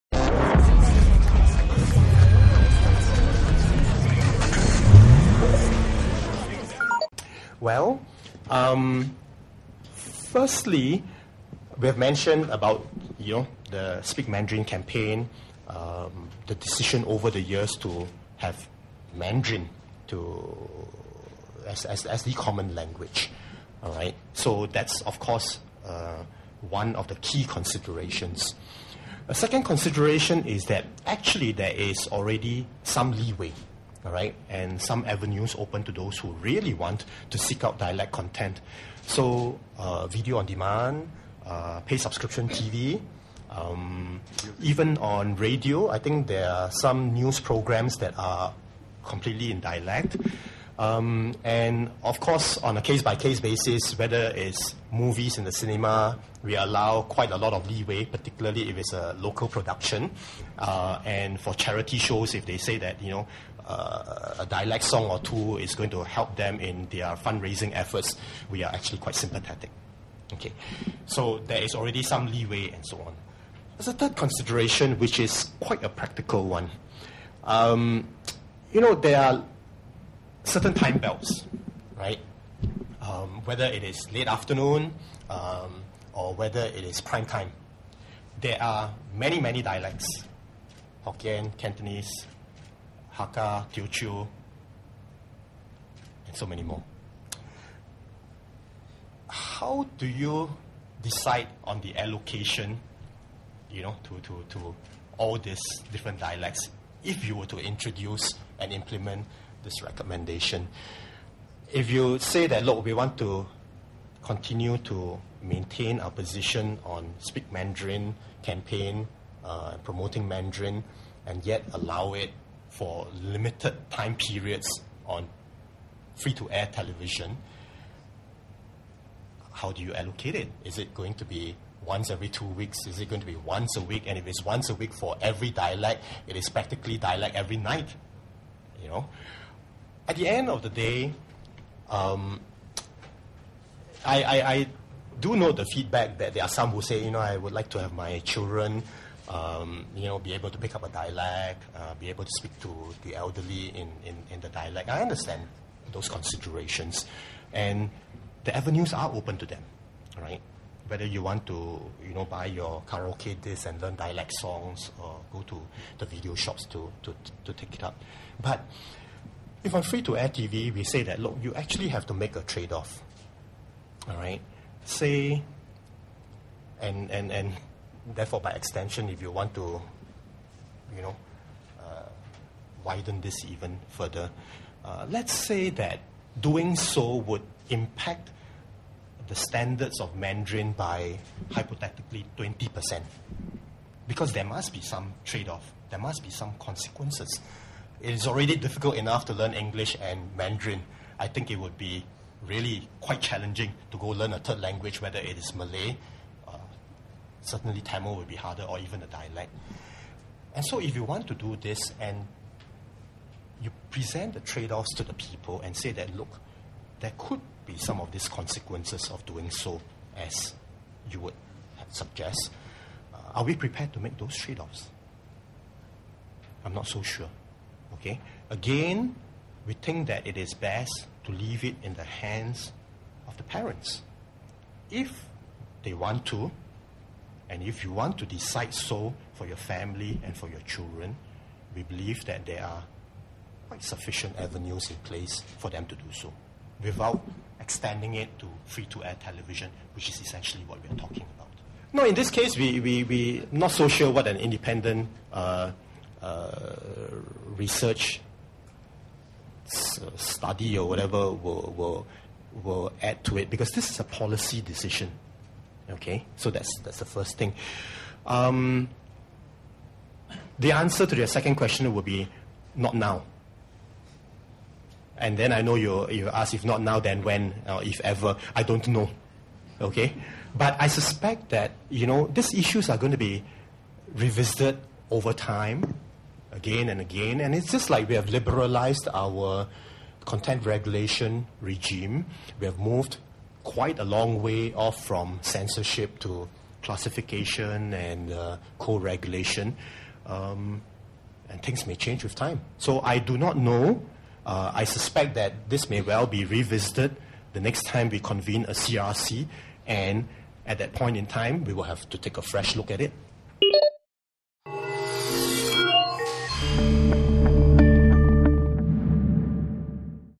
Dialect content in local news channels? (Censorship Review Part 7)
Dialect content in local news channels will not be increased. The reasons for not increasing the dialect content on local news channels was explained by Acting MICA Minister Lui Tuck Yew.